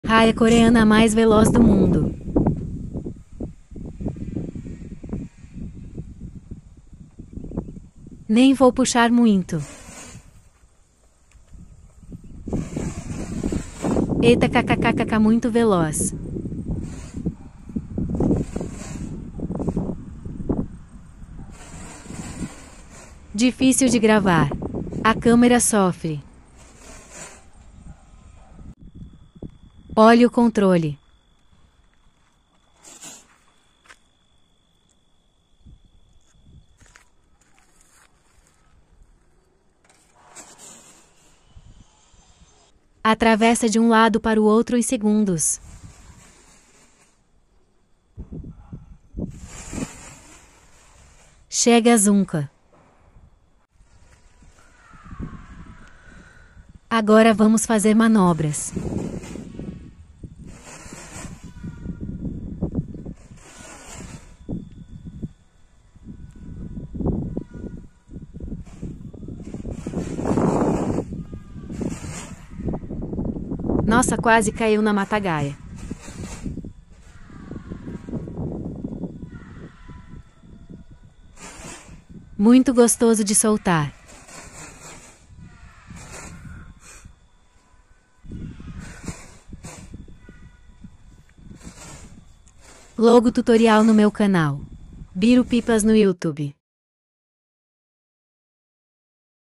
Raia mais veloz do mundo !!! deixa a Veloster na poeira 🤣🤣🧵
está raia e muito conhecida em campeonatos mundial por dar muito trabalho Para os oponente